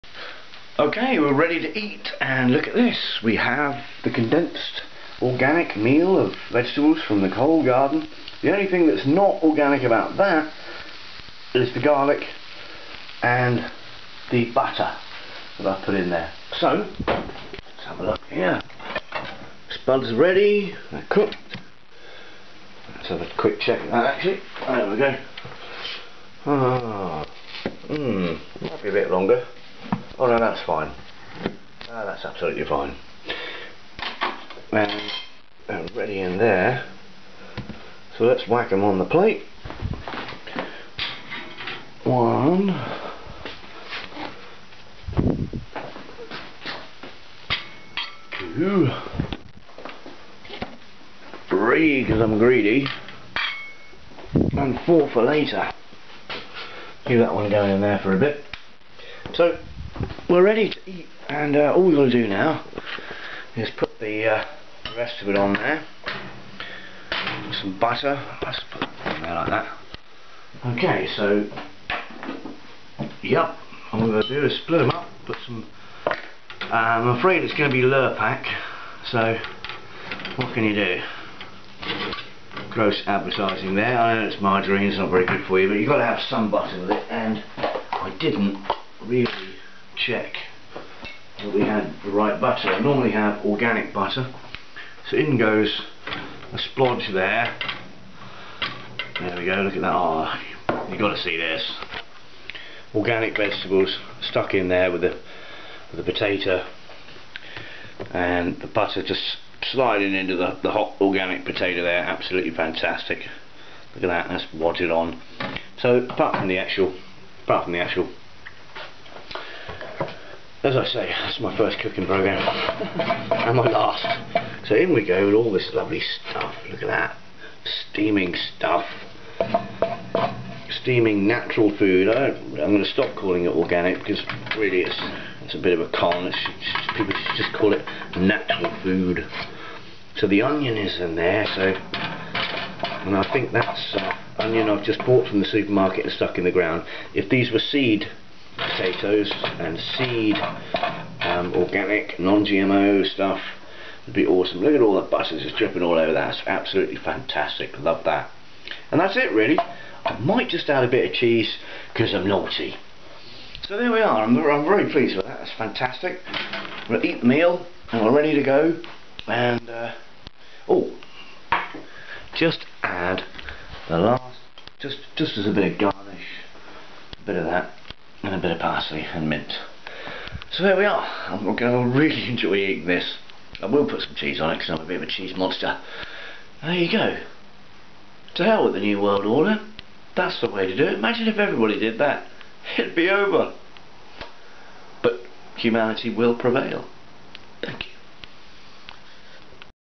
GROWGANIC GARDEN FOOD Pt3 FreeTruth Show
Nothing like real food from the garden. Apart from the Chemtrails and GMO origins of the spuds and onions, it's as natural as can be.